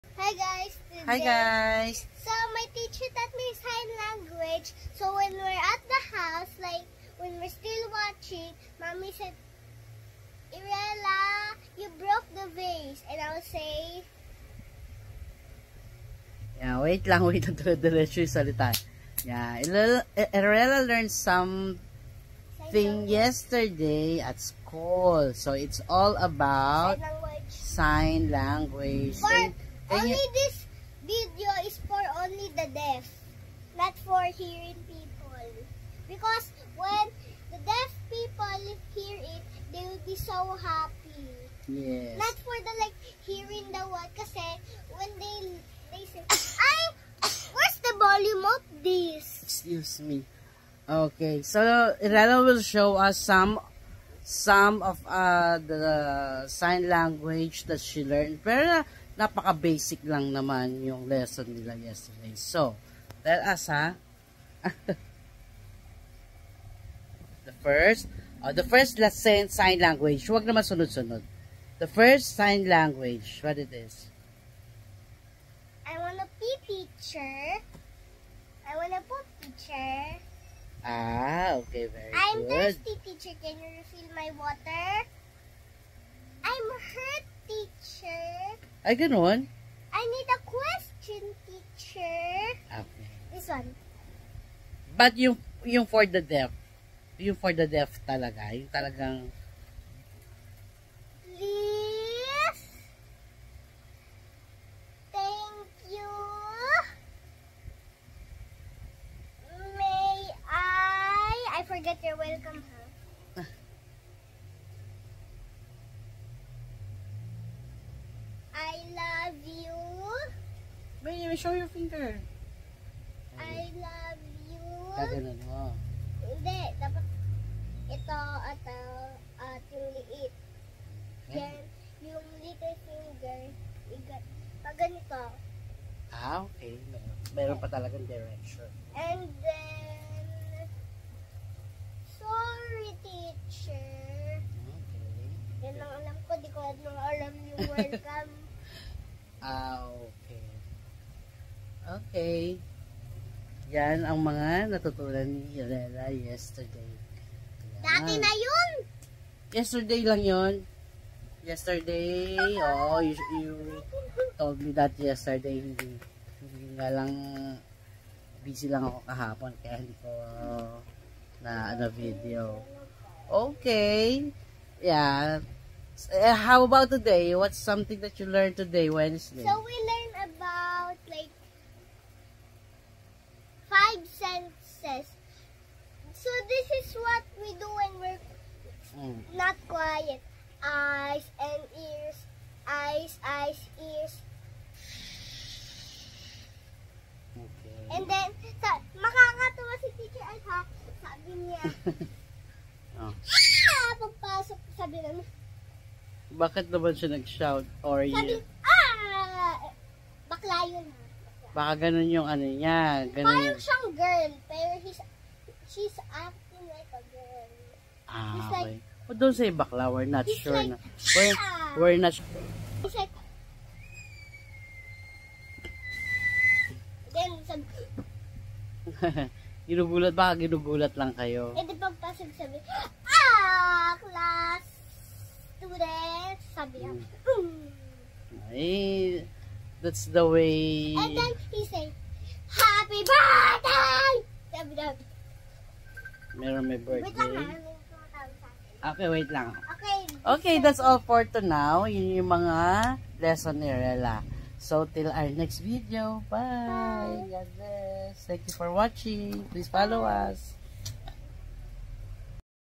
Sign language
8.15.24 💕 For today's video we have our kwentuhan about what Erelah learned in school. 🥰

Pls. watch and share our video. 🥰

シ